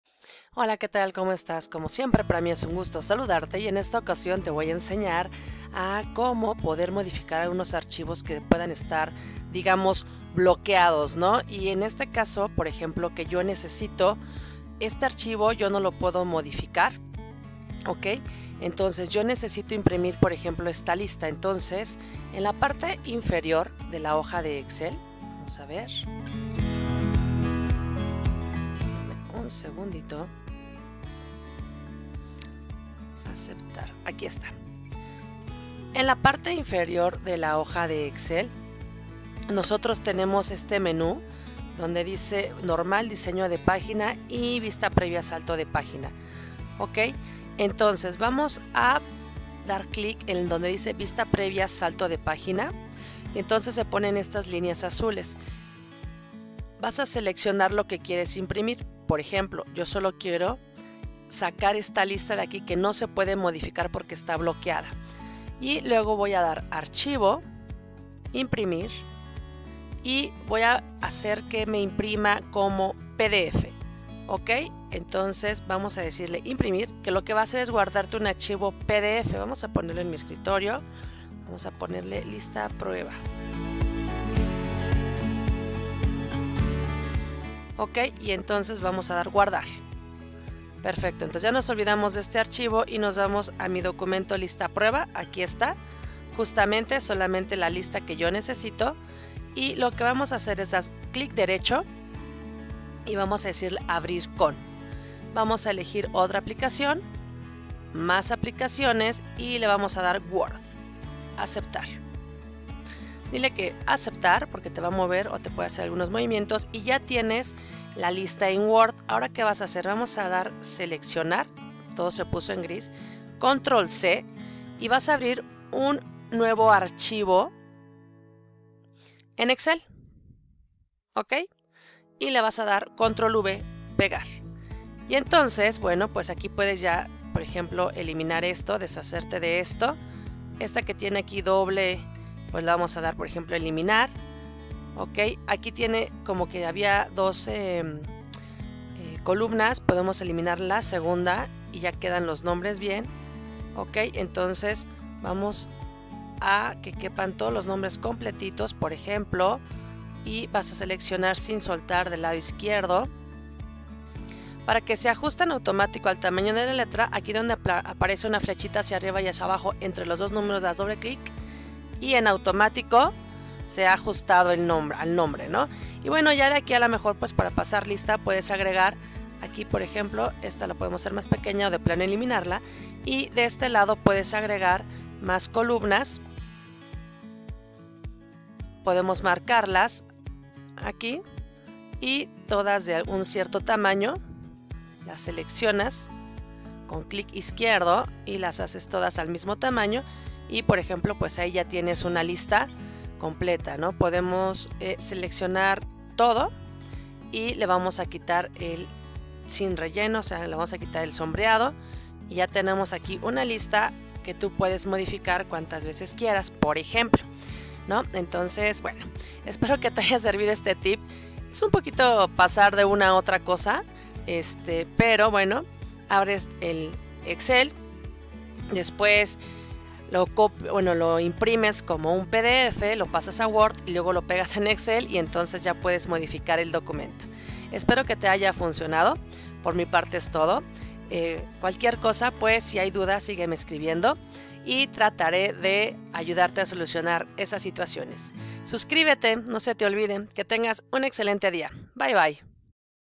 modificar excel bloqueado
Aprenderás a sacar información de un documento excel que esta bloqueado para que puedas trabajar